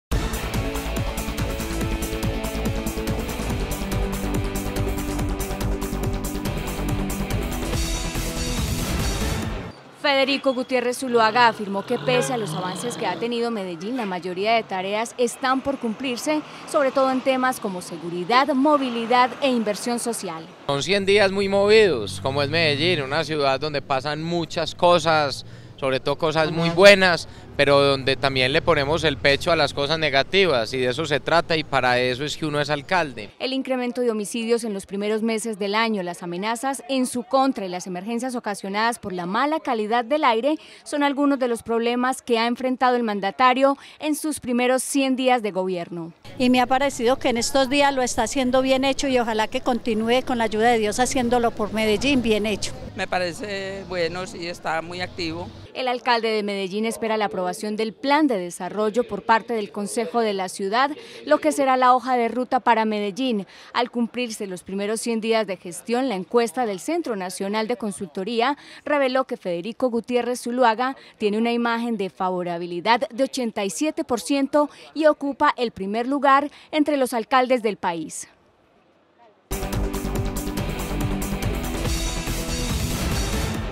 Alcalde de Medellín destacó que su enfoque es tener contacto con las problemáticas de la ciudad
En los 100 primeros días de gestión de Federico Gutiérrez, el alcalde de Medellín destacó que el enfoque de su trabajo haya sido recorrer las calles y tener contacto y cercanía con las problemáticas que vive la ciudad.